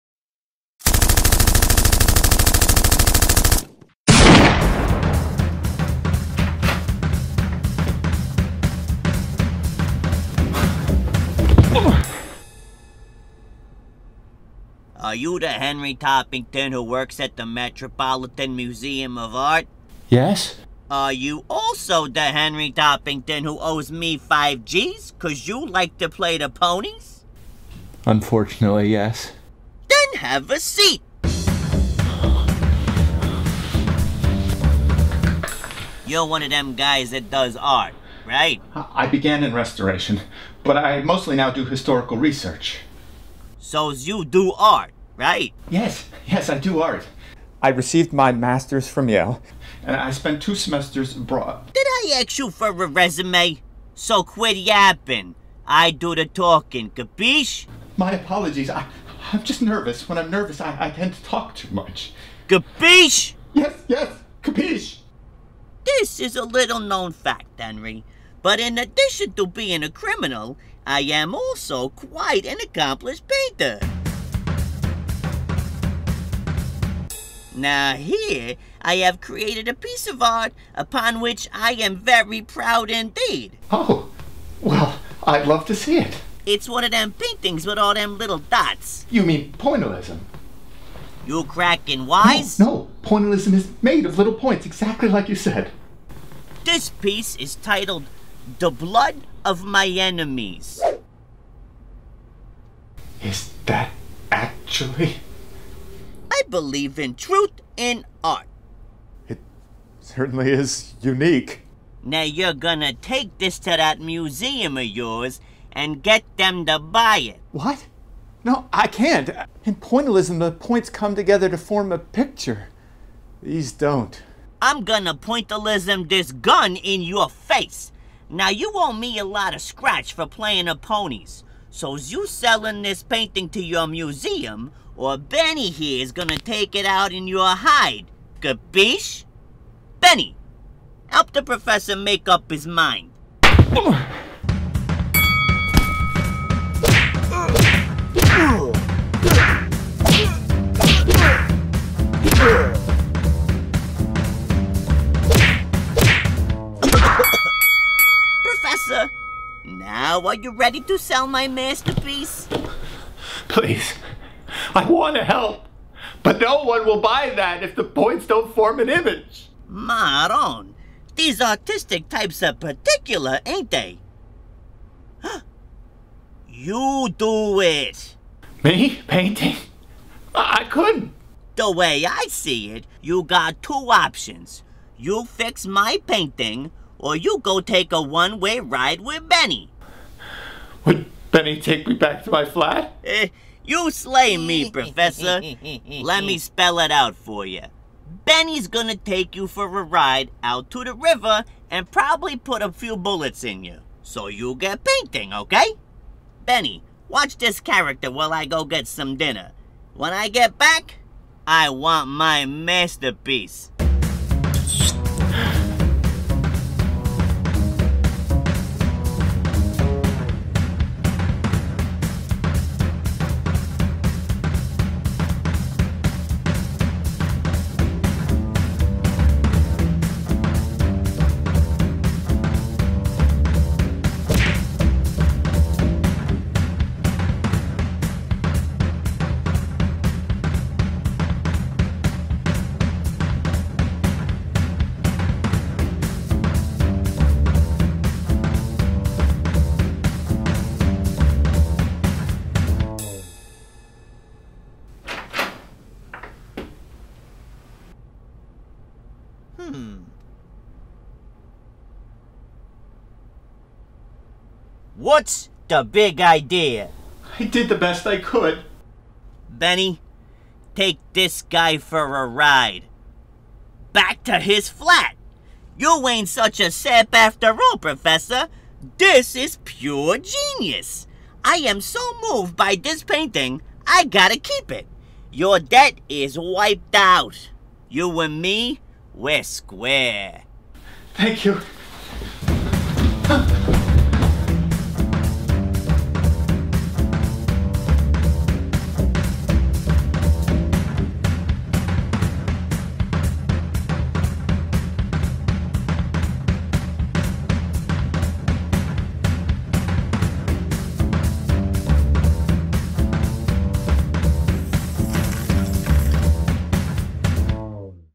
Pointillism- a Mouthpiece Marconi Film
Faced with a deadbeat Mouthpiece Marconi is forced to get creative.